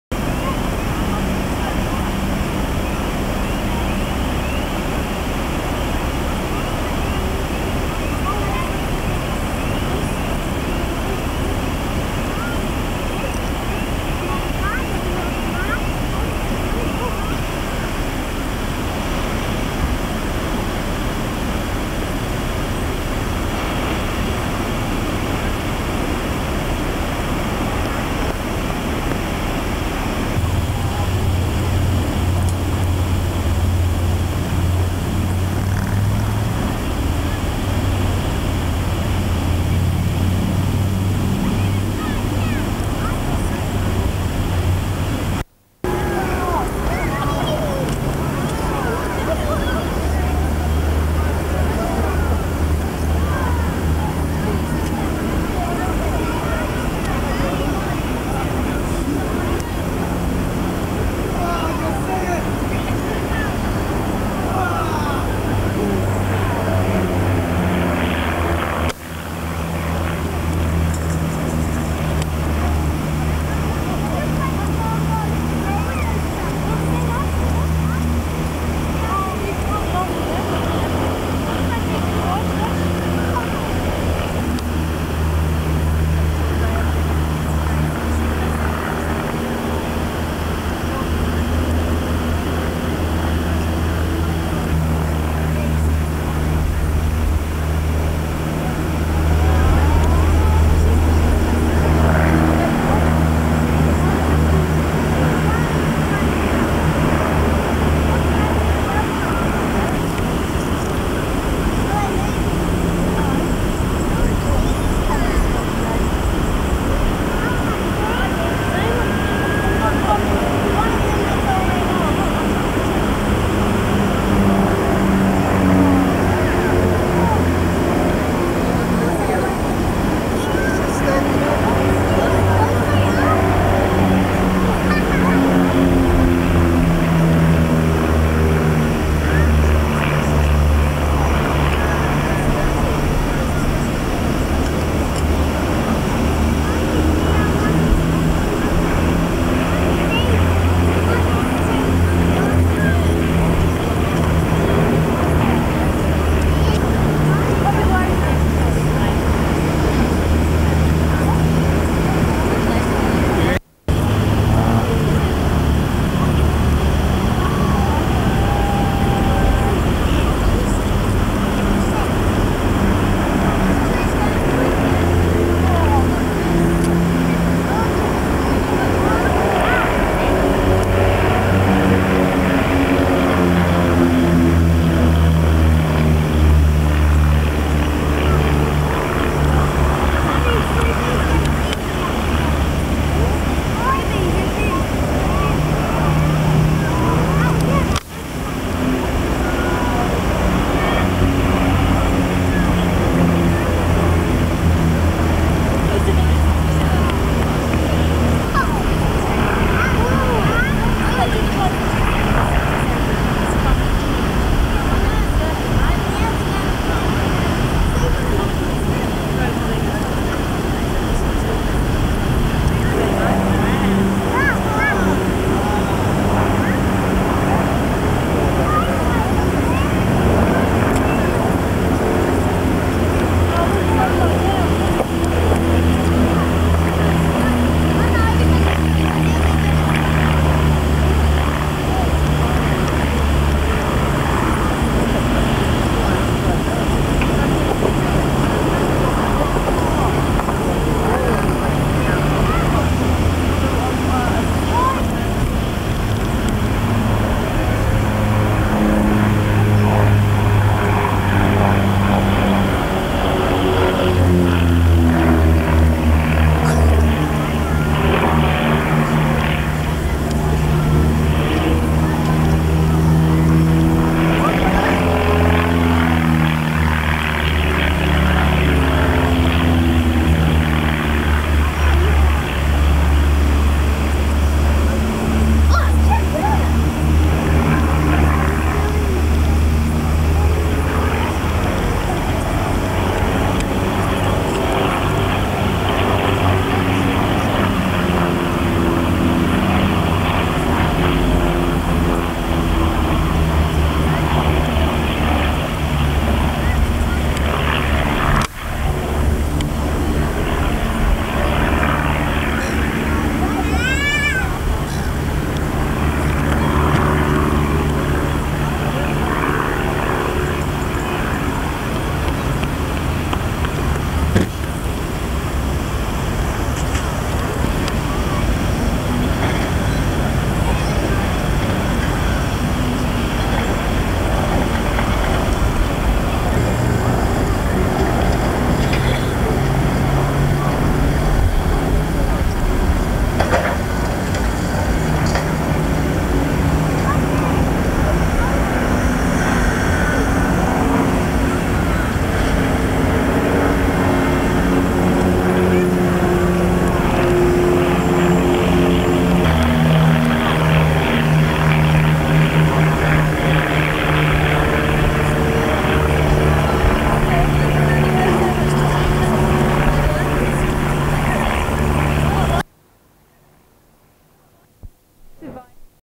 1993 Aerobatics Newcastle Beach
Pitts special Aerobatics display Newcastle beach 1993. By Phil Unicomb and Dave Freedman